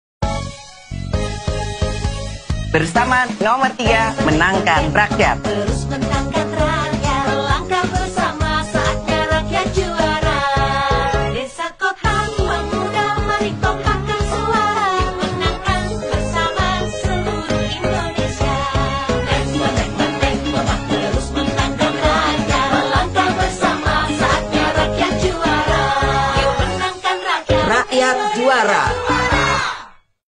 Menangkan Rakyat Bersama PDI Perjuangan!
Menangkan Rakyat Bersama PDI Perjuangan! PDI Perjuangan menegaskan bahwa memenangkan pemilu 2024 sama dengan memenangkan Rakyat.